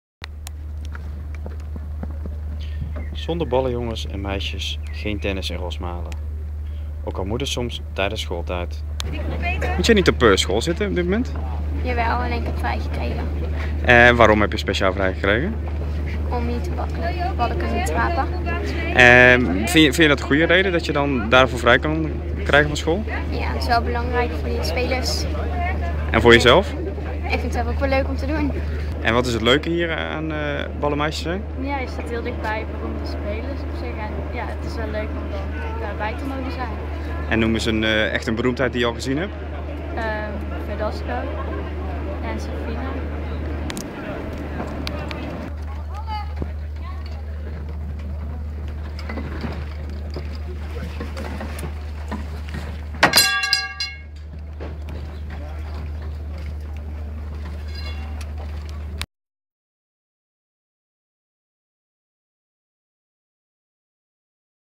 Ballen rapen op de Ordina Open
ROSMALEN - Zonder ballenjongens en -meisjes kan een groot tennistoernooi als de Ordina Open in Rosmalen niet bestaan. Zie ook